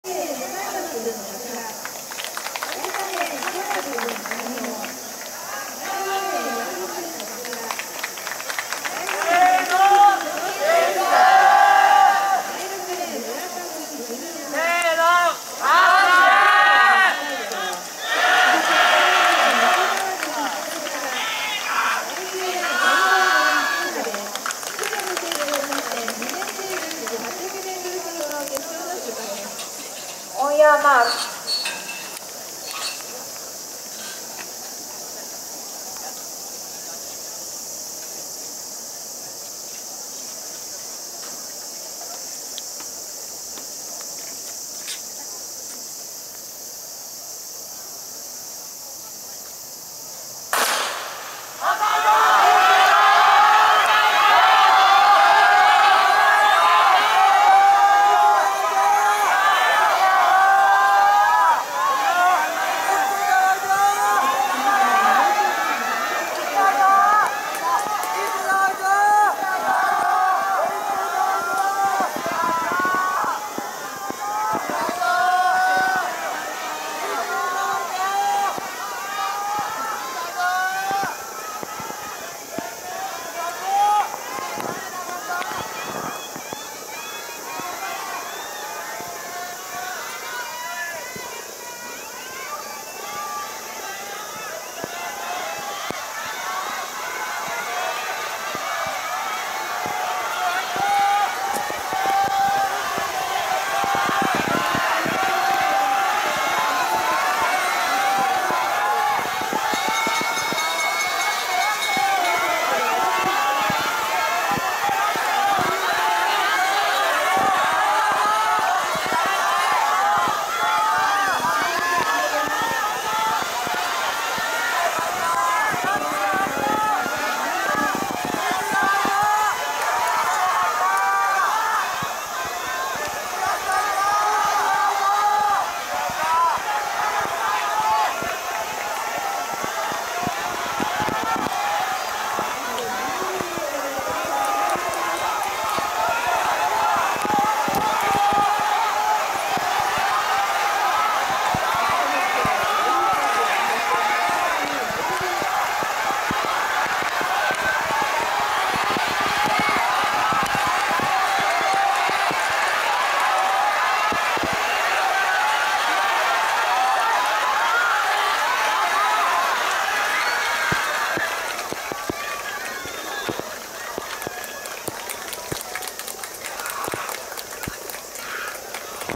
京都ユース選手権 男子2年800ｍ決勝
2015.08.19　　　　　　　　　　於：西京極運動公園陸上競技場
(1) 1:59.48 　鵜池　優至(2)　 [西京]
(2) 1:59.60 　西澤　燿(2)　 [北嵯峨]
(3) 1:59.62 　八木　祐太(2)　 [桂]
(4) 1:59.90 　村田　達哉(2)　 [久御山]
(5) 2:02.82 　向山　晃司(2)　 [桂]
(6) 2:03.01 　小藪　栞大(2)　 [網野]
(7) 2:03.23 　佐々木　一成(2)　 [京産大附]
(8) 2:04.13 　近藤　泰地(2)　 [田辺]